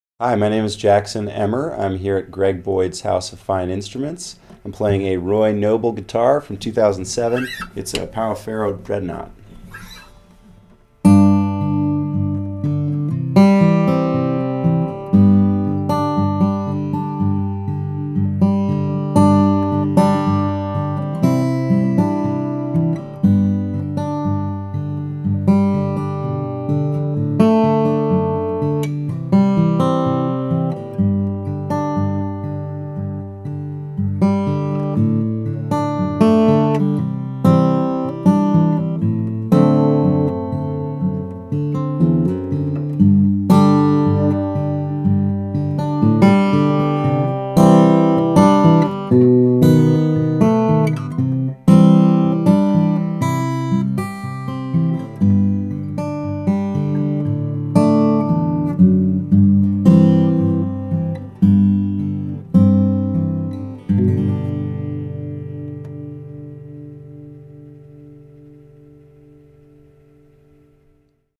2007 Roy Noble Pau Ferro Dreadnaught Guitar
Another beautiful guitar from Mr Noble! Pau Ferro back and sides. Bearclaw-figured Adirondack Red Spruce top. Ebony fingerboard and bridge. Waverly tuners. The body is bound with Maple outer bindings with Roy's handmade herringbone purflings inside the top edge and around soundhole rosette. Ebony fingerboard and bridge. No inlays in fingerboard. This guitar exhibits the usual Roy Noble tone, with quick attack, great balance, expression, and big projection that cuts through. As we've pointed out to many customers over the years, the Noble guitars don't project from a "forcefulness". They project due to tonal "purity" rather than with "muscle". They really are a treat to play and hear.